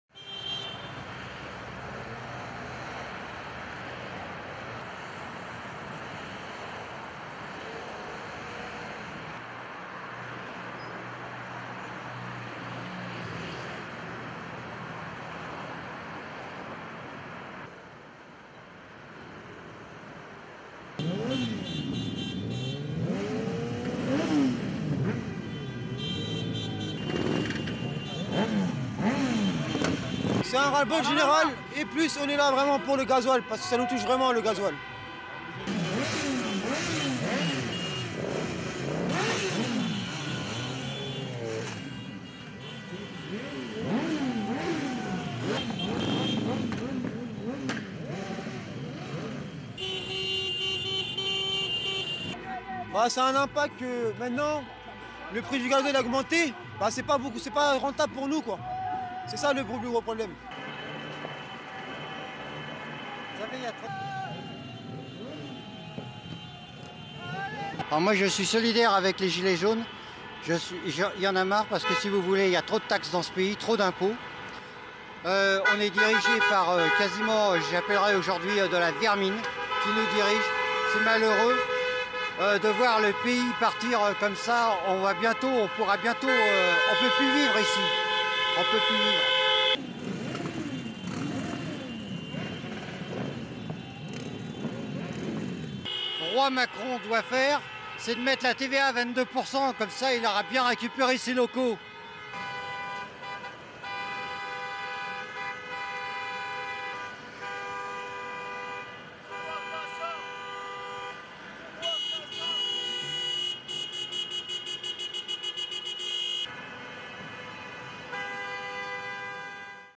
Thousands of French protesters gather over fuel tax rise
French officials confirmed the woman was killed after a driver accelerated as demonstrators blocked roads during a fuel tax protest. The motorist accidentally hit and killed the protester taking part in a campaign of road blockades across France today as thousands gathered on motorways in a backlash against President Emmanuel Macron's planned higher fuel taxes. The woman was said to have been attending a protest in the Savoie region of the Auvergne-Rhone-Alpes area in the south east of the country when the accident happened.